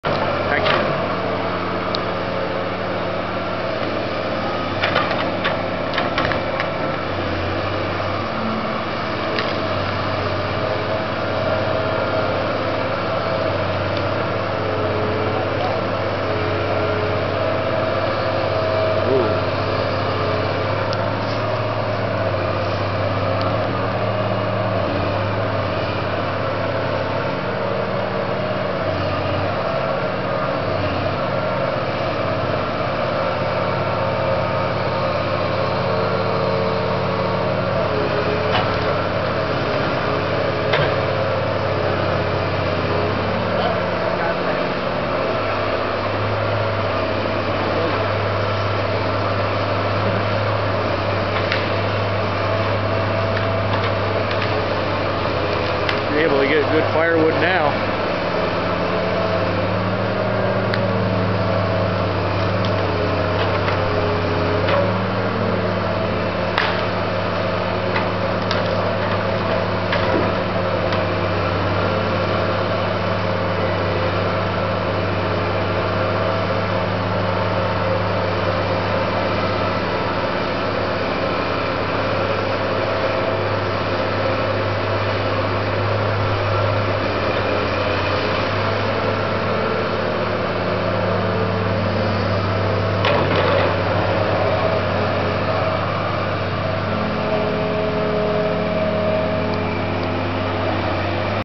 LOGGING LOADER IN ACTION
One of the guys was moving the deadly Hemlock tree with that big ol' machine. Filmed on 1/18/10.